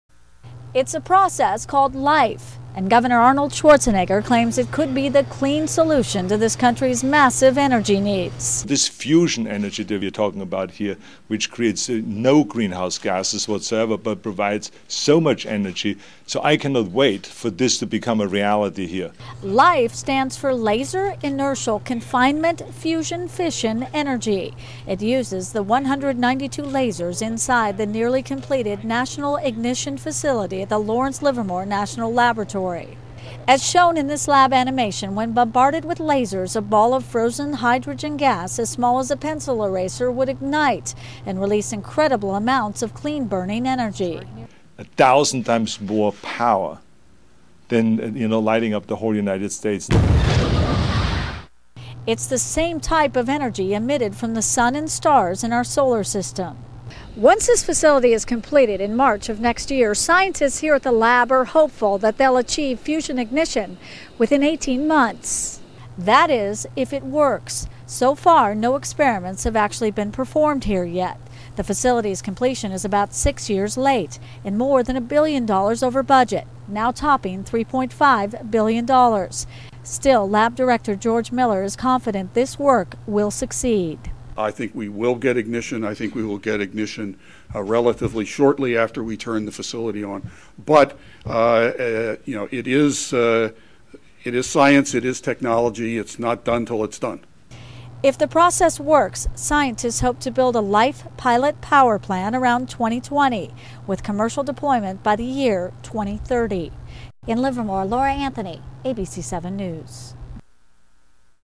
Gov. Schwarzenegger talks clean fusion energy
Arnold Schwarzenegger: Nuclear fusion power breakthrough
California lab says it's found a way to create an endless supply of clean energy. 11/11/2008